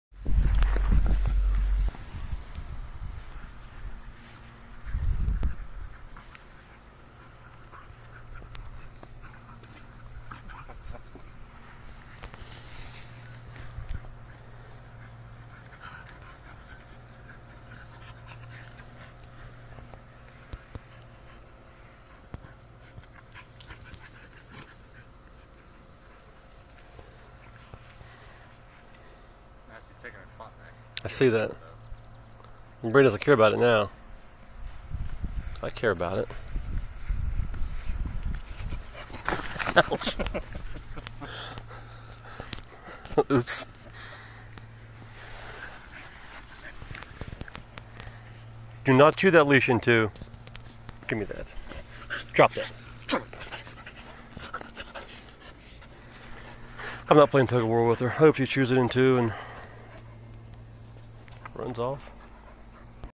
Lex the rescue dog Maniac
She loves running around the yard like a maniac.